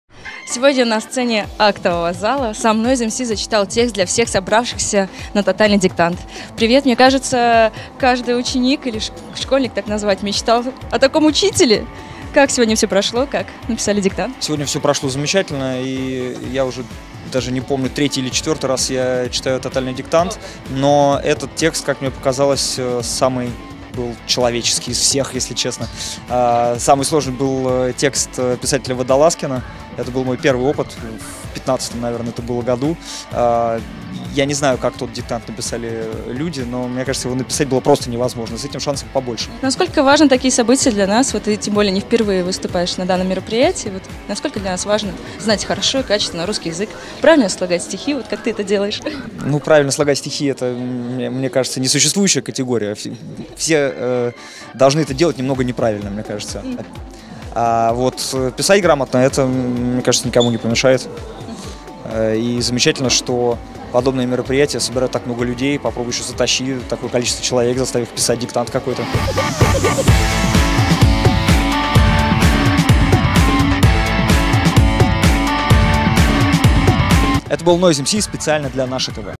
Noize MC - Интервью для НАШЕ ТВ после Тотального Диктанта (14.04.2018)
Тота́льный дикта́нт — ежегодное образовательное мероприятие, организуемое с 2004 года в России и разных странах мира с целью популяризации грамотности. Представляет собой диктант, который читают известные деятели культуры для всех желающих проверить свои знания.